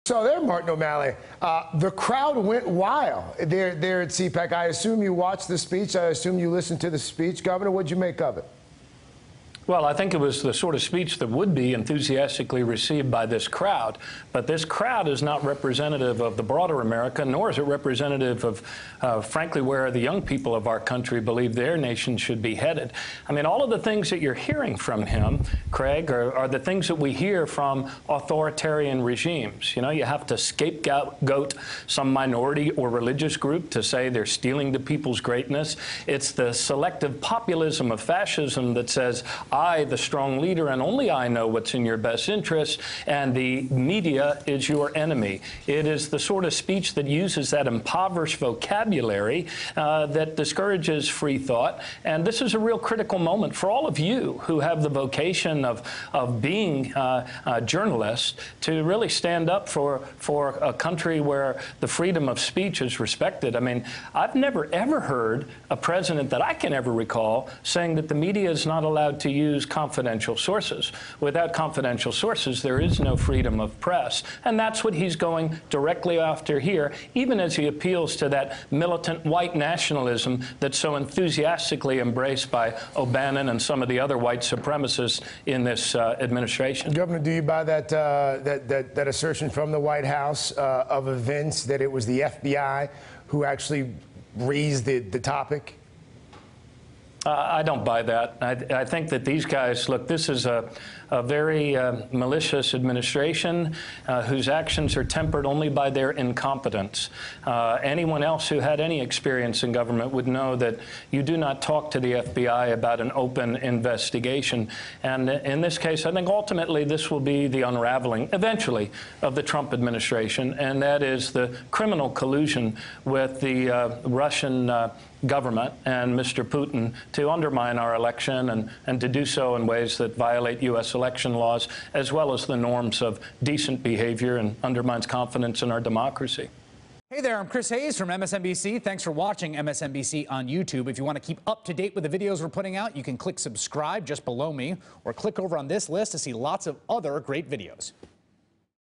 Martin O'Malley: Trump Administration 'Malicious' Tempered Only By Incompetence | MSNBC
Governor Martin O'Malley joins MSNBC's Craig Melvin to explain how President Trump appealed to "militant White nationalism" during his CPAC speech, and speculates that the Trump administration is on the verge of "unraveling."